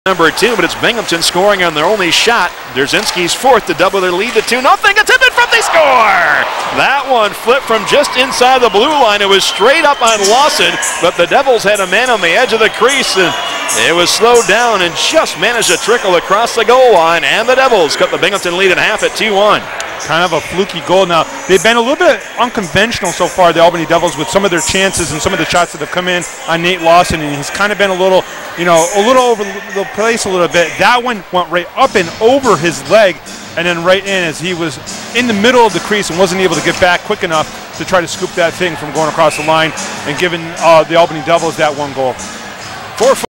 4/12/2013 Steven Zalewski Goal
Steven Zalewski deflects a shot past Nathan Lawson. Assists to Jay Leach and Eric Gelinas.